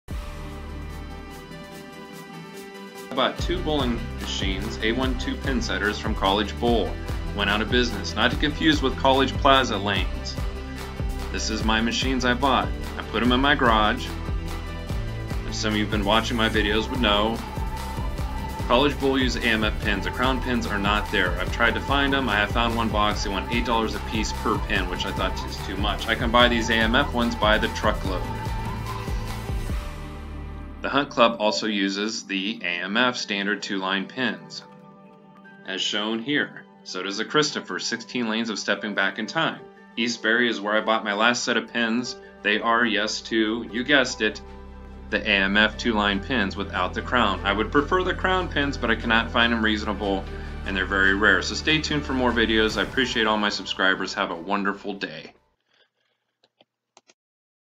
Why I do not use crown pins
In the comments of one of my videos I was asked why I don't use crown pins? This is why...